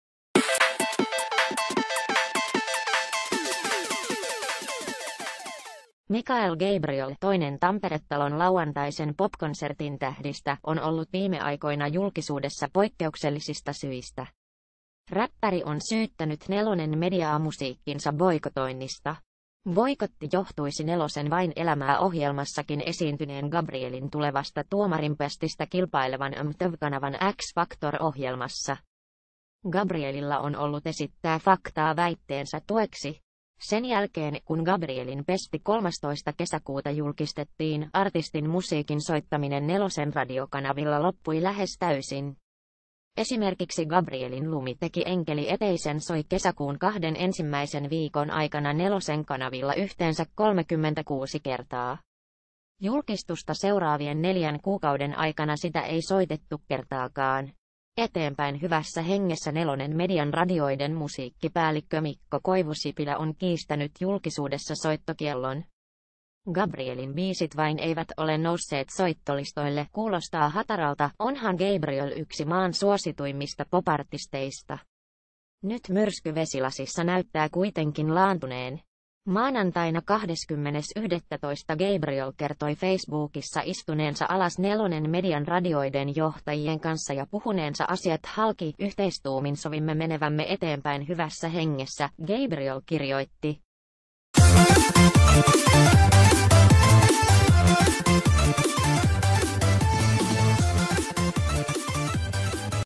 Boikottikohu on ohi – mikael gabriel konsertoi lauantaina tampere-talossa isac elliotin kanssa
{Sisällön 200}
{Otsikko} Kuvien ja äänten käyttävät videot eivät kuulu tekijänoikeuksieni. Jos se on sinun. haluat poistaa sen.